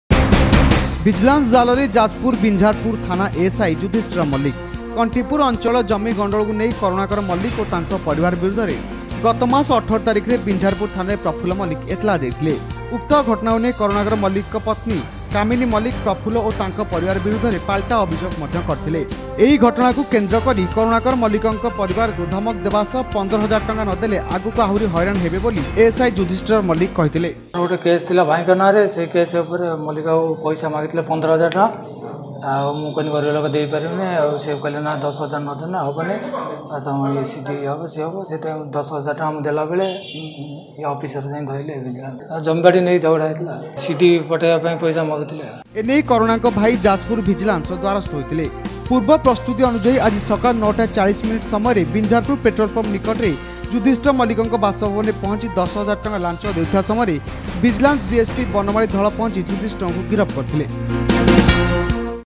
ଭିଜିଲାନ୍ସ ଜାଲରେ ସରକାରୀ ବାବୁ
ଭିଜିଲାନ୍ସ ଜାଲରେ ଯାଜପୁର ବିଂଝାରପୁର ଥାନା ଏସ.ଏସ.ଆଇ ଯୁଧିଷ୍ଟିର ମଲ୍ଲିକ । କଂଟିପୁର ଅଂଚଳର ଜମି ଗଣ୍ଡଗୋଳକୁ ନେଇ କରୁଣାକର ମଲ୍ଲିକ ଓ ତାଙ୍କ ପରିବାର ବିରୁଦ୍ଧରେ ଗତ ମାସ ୧୮ ତାରିଖରେ ବିଂଝାରପୁର ଥାନାରେ ପ୍ରଫୁଲ୍ଲ ମଲ୍ଲିକ ଏତେଲା ଦେଇଥିଲେ । ଉକ୍ତ ଘଟଣାକୁ ନେଇ କରୁଣାକର ମଲ୍ଲିକଙ୍କର ପତ୍ନୀ କାମିନୀ ମଲ୍ଲିକ ପ୍ରଫୁଲ୍ଲ ଓ ତାଙ୍କ ପରିବାର ବିରୁଦ୍ଧରେ ପାଲଟା ଅଭିଯୋଗ କରିଥିଲେ । ଏହି ଘଟଣାକୁ କେନ୍ଦ୍ରକରି କରୁଣାକର ମଲ୍ଲିକଙ୍କ ପରିବାରକୁ ଧମକ ଦେବାସହ ୧୫ ହଜାର ଟଙ୍କା ନଦେଲେ  ଆଗକୁ ଆହୁରୀ ହଇରାଣ ହେବେ ବୋଲି ଏ.ଏସ.ଆଇ ଯୁଧିଷ୍ଟିର ମଲ୍ଲିକ କହିଥିଲେ । ଏନେଇ କରୁଣାଙ୍କ ଭାଇ ଯାଜପୁର ଭିଜିଲାନ୍ସର ଦ୍ୱାରସ୍ଥ ହୋଇଥିଲେ  । ପୂର୍ବ ପ୍ରସ୍ତୁତି ଅନୁଯାୟୀ ଆଜି ସକାଳ ୯.୪୦ ମିନିଟ୍ ସମୟରେ ବିଂଝାରପୁର ପେଟ୍ରୋଲ ପମ୍ପ ନିକଟରେ ଯୁଧିଷ୍ଟିର ପ୍ରଧାନଙ୍କ ବାସଭବନରେ ପହଂଚି ୧୦ ହଜାର ଟଙ୍କା ଲାଂଚ ଦେଉଥିବା ସମୟରେ ଭିଜିଲାନ୍ସ  ଡିଏସପି ବନମାଳୀ ଧଳ ପହଂଚି ଯୁଧିଷ୍ଟିରଙ୍କୁ ଗିରଫ କରିଥିଲା ।